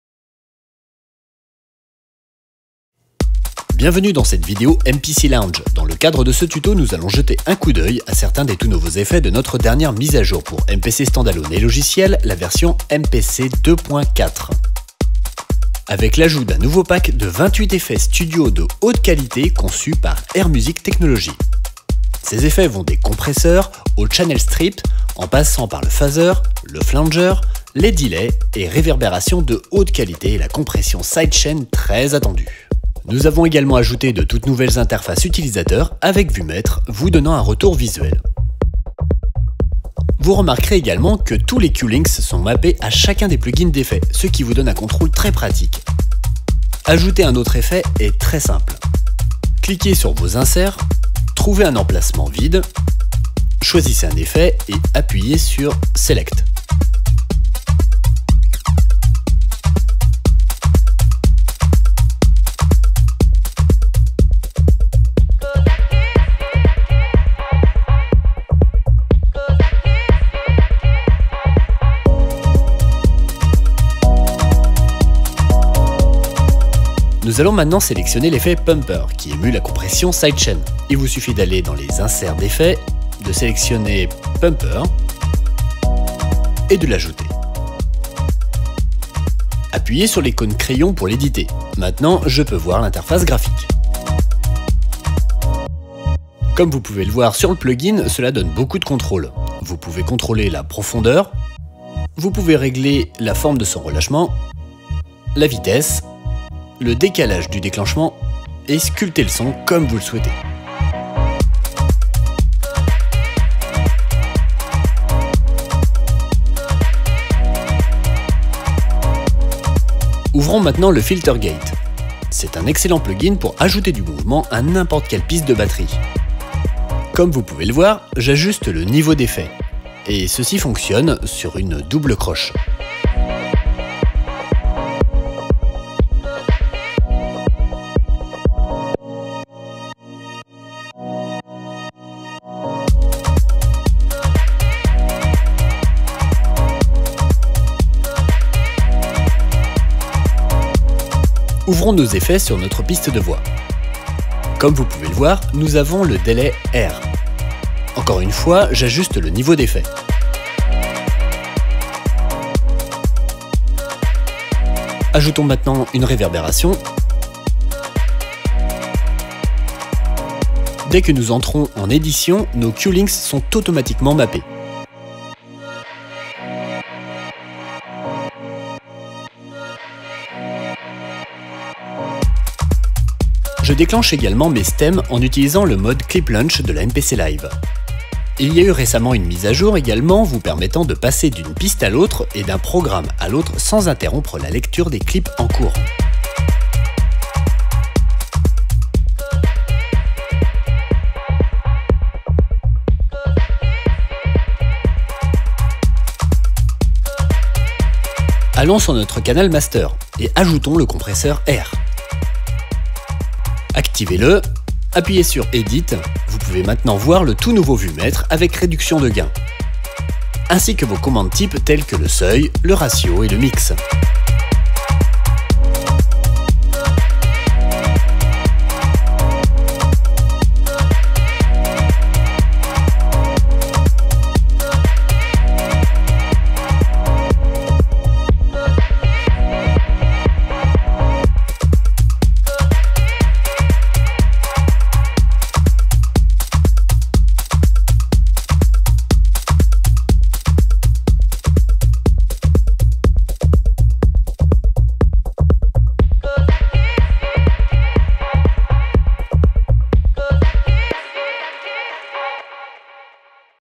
AKAI MPC 2.4 : tour d'horizon des effets (vidéo de La Boite Noire)
Vidéo tuto autour des effets sur la version MPC 2.4 de la MPC LIVE AKAI.

Les stations de production autonomes AKAI MPC LIVE et MPC X sont régulièrement mises à jour.

FICHE TECHNIQUE
- MPC autonome (pas d'ordinateur requis)
- Administré par le logiciel MPC Software 2.0
- Écran couleur multitouch 7"
- Également utilisable comme surface de contrôle sur Mac et PC
- Batterie lithium-ion rechargeable offrant jusqu'à 5 heures d'autonomie
- Entrées sélectionnables : platine vinyle (Rca) avec connecteur de masse et jack 6,35 mm TRS
- Sortie casque 3,5 mm
- 2 entrées et 2 sorties Midi
- 2 Go de mémoire vive
- 16 Go de stockage intégré (dont plus de 10 Go de contenu sonore premium de sound designers tels que CR2, Toolroom, Loopmasters, Capsun Pro Audio et bien plus encore)
- Connecteur Sata 2,5'' (SSD ou HDD)
- 2 ports USB-A 3.0 pour clé USB ou contrôleurs Midi
- Lecteur de carte SD pour charger des échantillons ou des projets
- Ableton Link via wifi
- Midi via Bluetooth 4.0